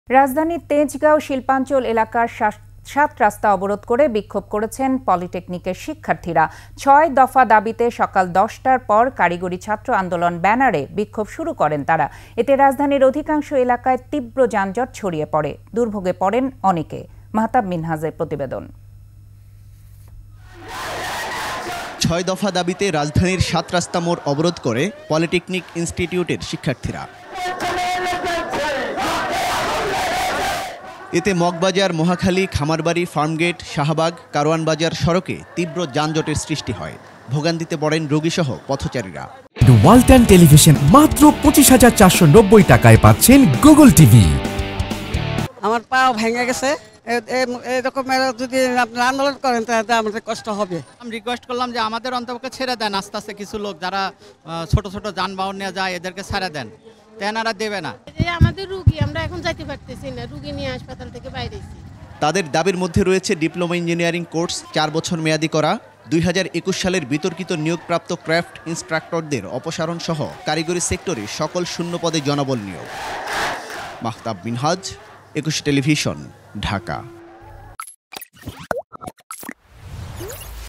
সাতরাস্তা অবরোধ করে বিক্ষোভ করেছেন পলিটেকনিকের শিক্ষার্থীরা || ETV News
রাজধানীর তেজগাঁও শিল্পাঞ্চল এলাকার সাতরাস্তা অবরোধ করে বিক্ষোভ করেছেন পলিটেকনিকের শিক্ষার্থীরা। ছয় দফা দাবিতে সকাল ১০টার পর ‘কারিগরি ছাত্র আন্দোলন’ ব্যানারে বিক্ষোভ শুরু করেন তারা। এতে রাজধানীর অধিকাংশ এলাকায় তীব্র যানজট  ছড়িয়ে পড়ে। দুর্ভোগে পড়েন অনেকে। মাহতাব মিনহাজের প্রতিবেদন।